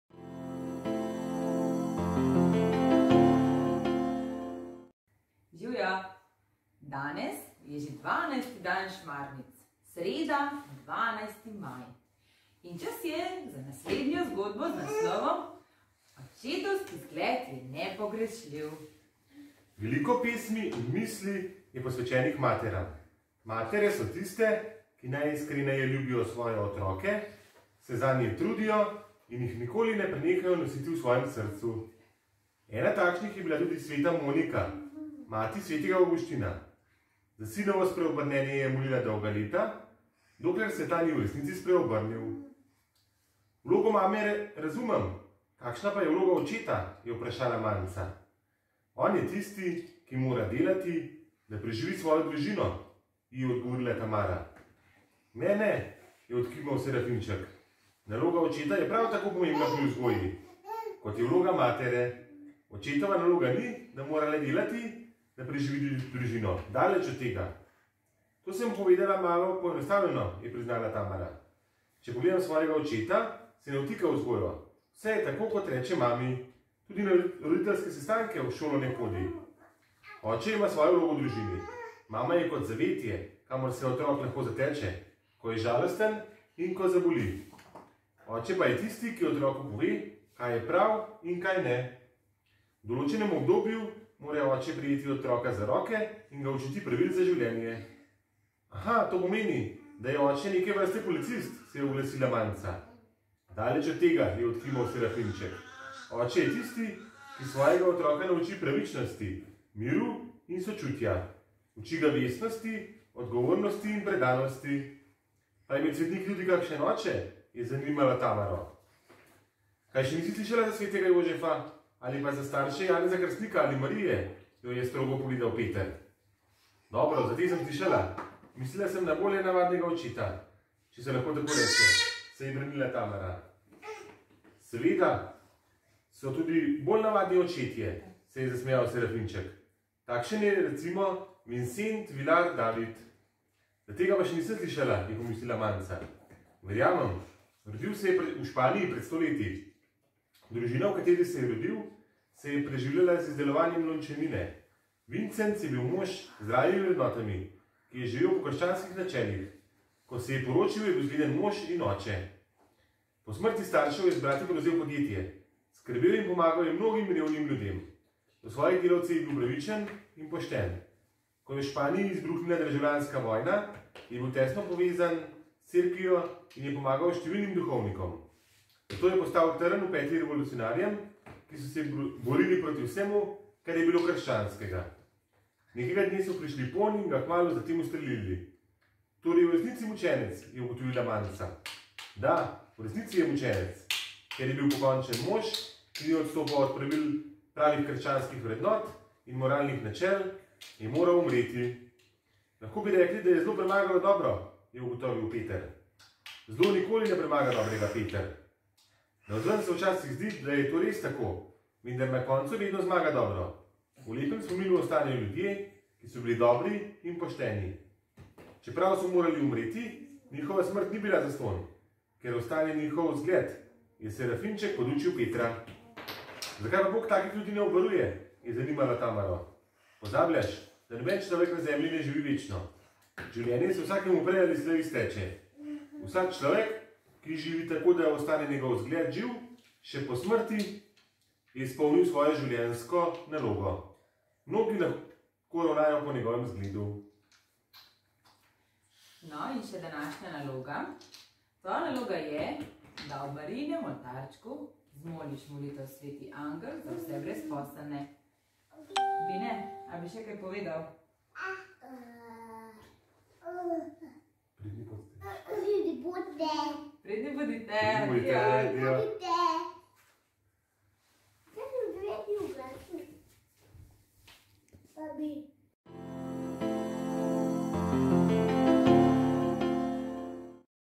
Šmarnice 2021 (12. dan)
Tudi letošnje leto bodo šmarnice potekale podobno kot lani, preko spleta.
Naslov letošnjih šmarnic je Svetniki so bili čisto (ne)navadni ljudje.
V sodelovanju z Družinskim in Mladinskim centrom Cerklje pri pripravi šmarnic sodelujejo družine in posamezniki iz župnije.